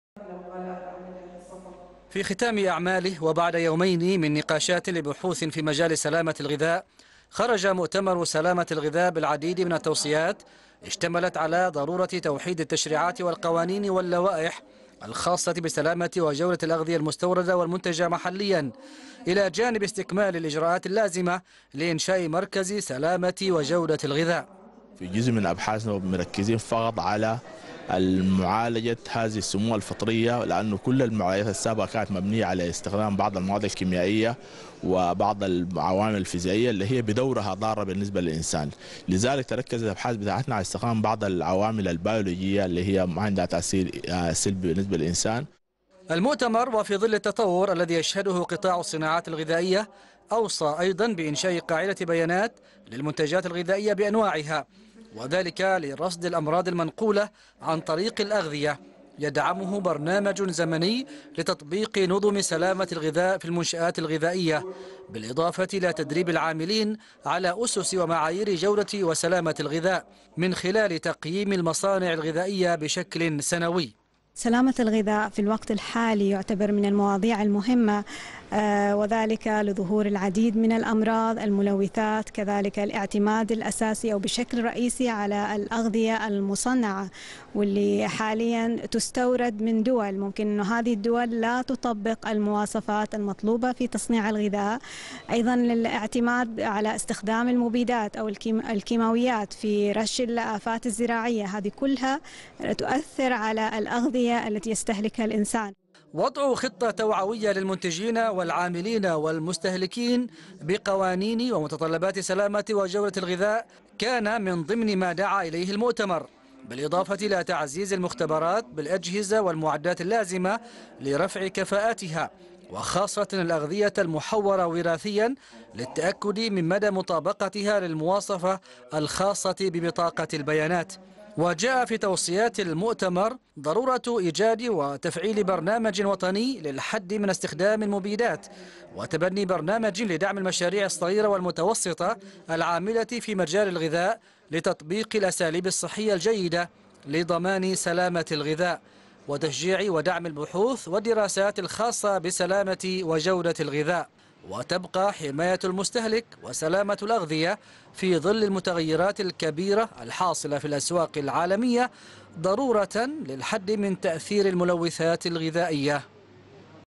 مؤتمر سلامة الغذاء يوصي في ختام أعماله بمراجعة وتوحيد التشريعات والقوانين بسلامة وجودة الأغذية
تقرير: بدر بن سعيد الشعيبي
قراءة: صالح بن حمد الزكواني